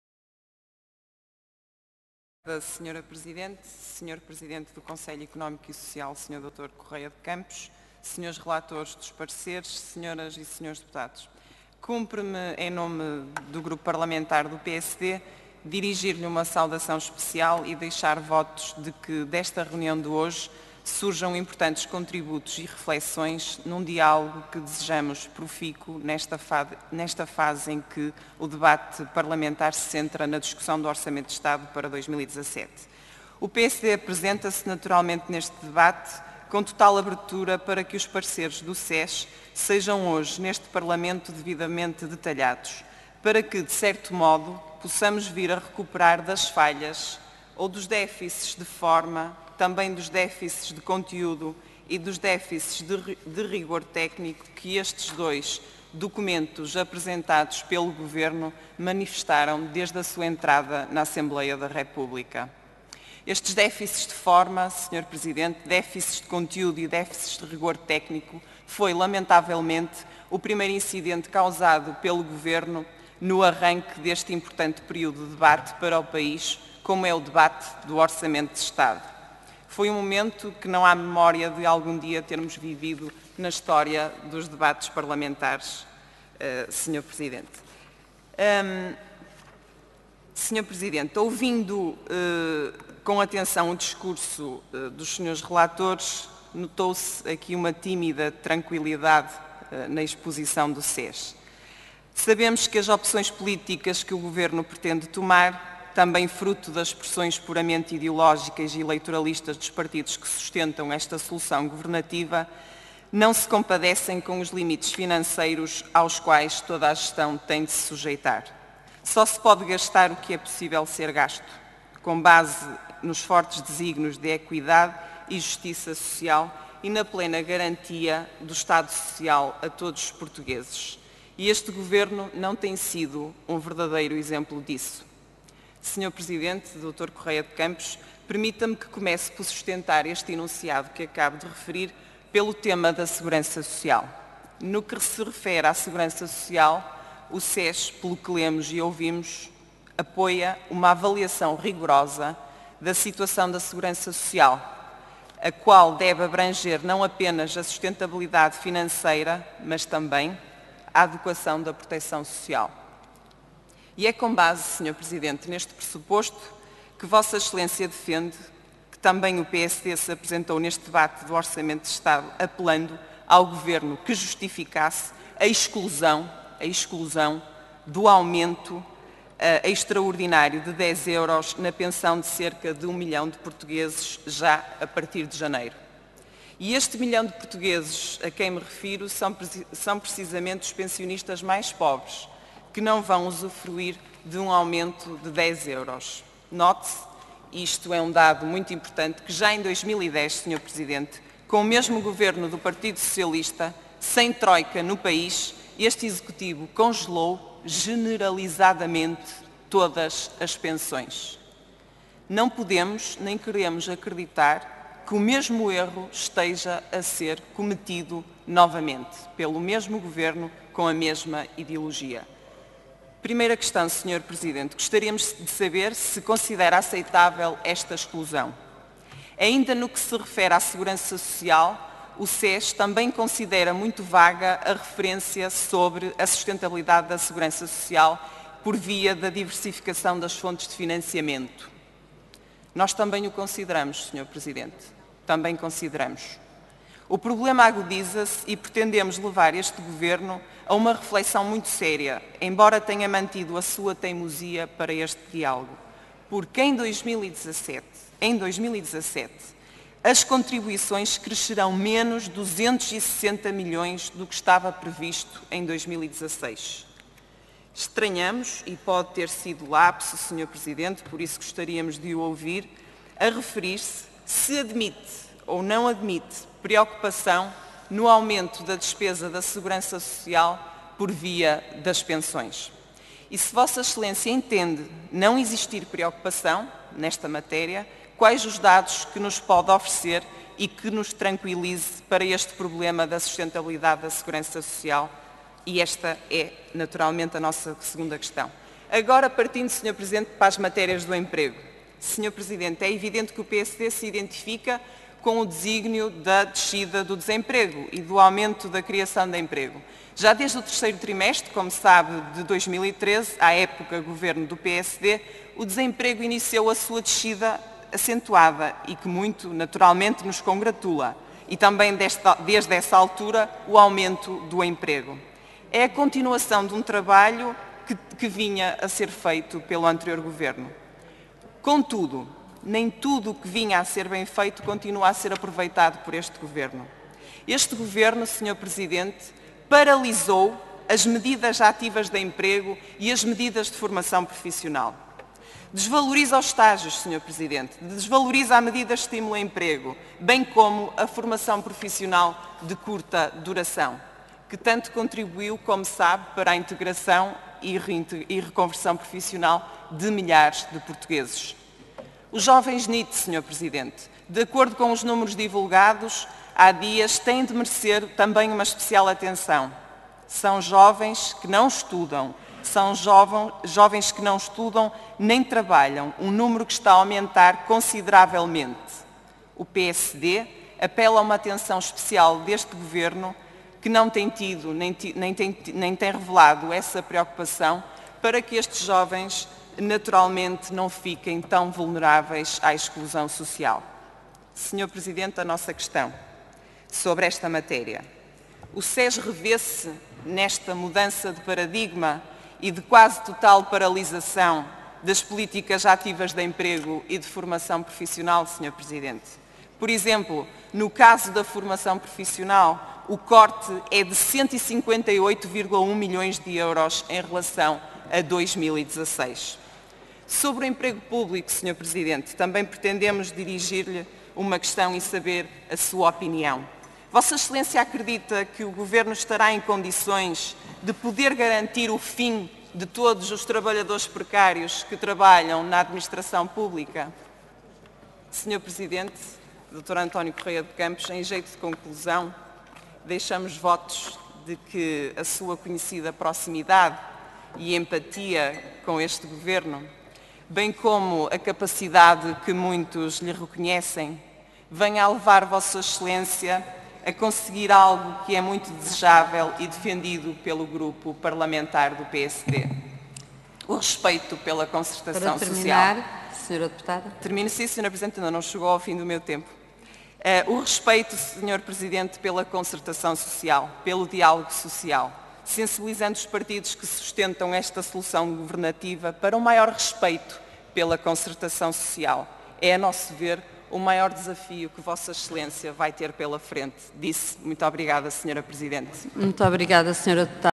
Carla Barros questiona Presidente do Conselho Económico e Social
Intervenção no âmbito da apreciação, na especialidade, das Propostas de Lei do Orçamento do Estado para 2017 e das Grandes Opções do Plano para 2017.